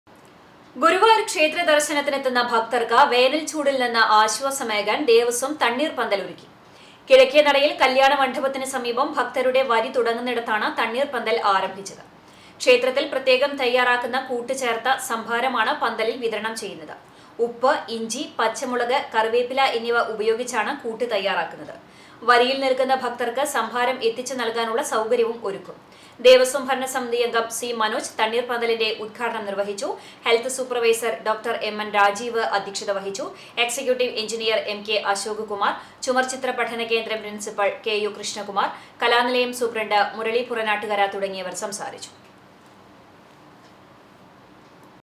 വേനല്‍ചൂടില്‍ നിന്ന് ആശ്വാസമേകാന്‍ തണ്ണീര്‍ പന്തല്‍ ഒരുക്കി ഗുരുവായൂര്‍ ദേവസ്വം
ഗുരുവായൂര്‍ ക്ഷേത്രദര്‍ശനത്തിനെത്തുന്ന ഭക്തര്‍ക്ക് വേനല്‍ചൂടില്‍ നിന്ന് ആശ്വാസമേകാന്‍ ദേവസ്വം തണ്ണീര്‍ പന്തല്‍ ഒരുക്കി. കിഴക്കേനടയില്‍ കല്യാണമണ്ഡപത്തിന് സമീപം ഭക്തരുടെ വരി തുടങ്ങുന്നിടത്താണ് തണ്ണീര്‍പന്തല്‍ ആരംഭിച്ചത്. ക്ഷേത്രത്തില്‍ പ്രത്യകം തയ്യാറാക്കുന്ന കൂട്ട് ചേര്‍ത്ത സംഭാരമാണ് പന്തലില്‍ വിതരണം ചെയ്യുന്നത്.